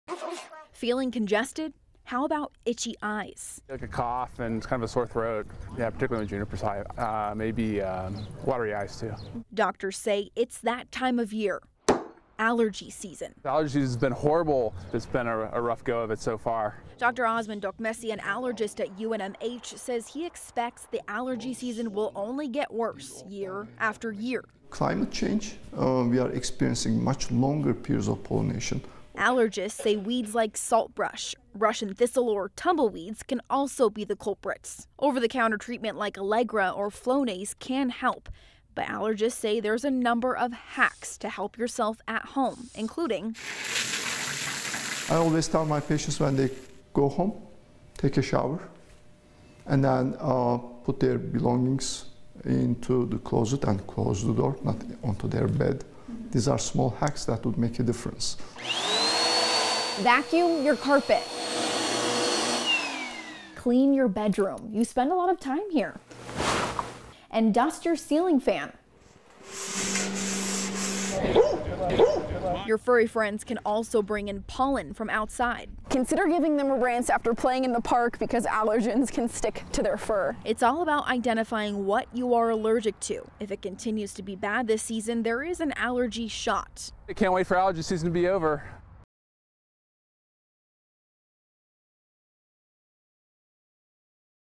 How to ease your suffering during allergy season
Doctors warn that climate change is making allergy season longer, and there are ways to ease your suffering without medication.